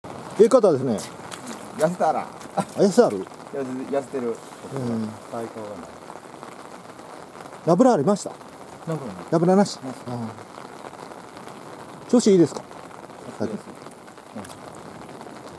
ルアーにヒットした青物魚ブリ 和歌山釣太郎
今朝は３投目でヒットした１本目の青物魚。３月２１日水曜日みなべ町岩代堤防。